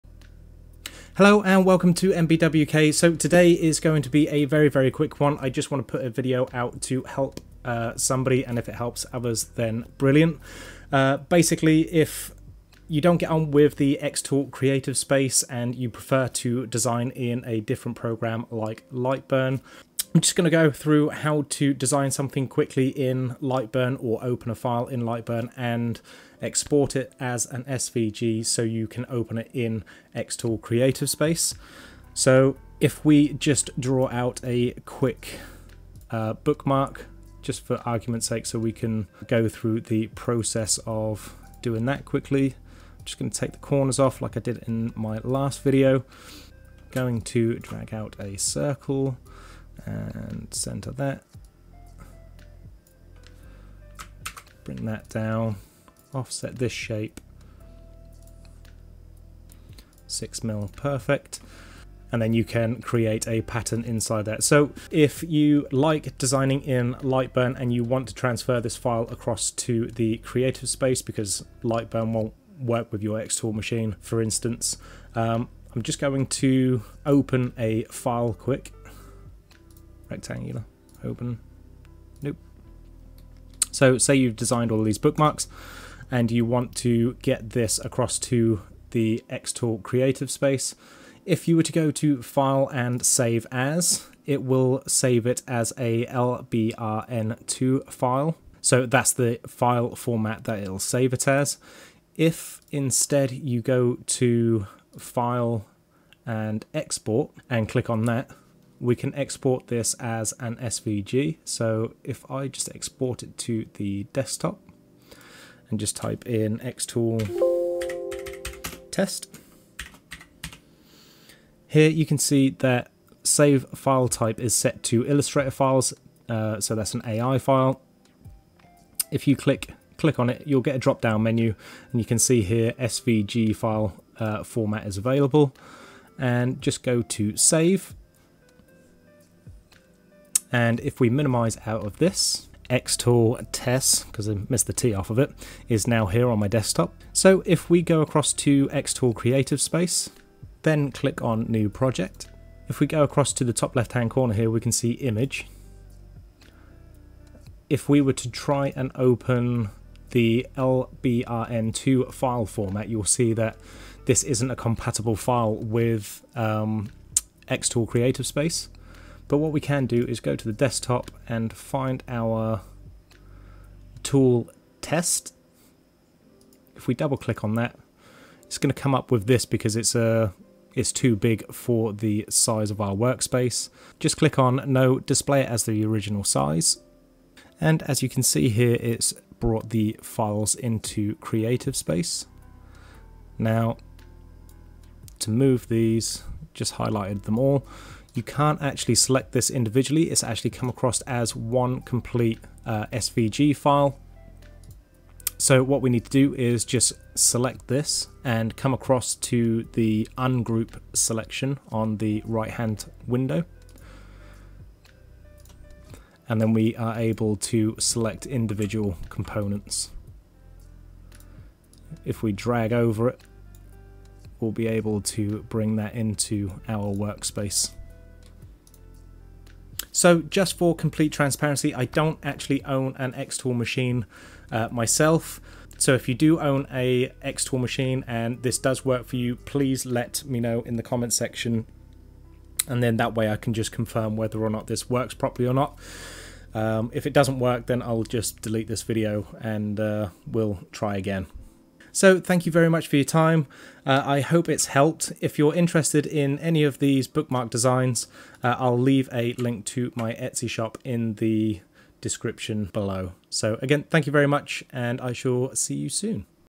Lightburn To Xtool Creative Space File Transfer. Open SVG In Xtool Creative Space
I have made this video to try to help someone with a specific problem. I hope this will help many more.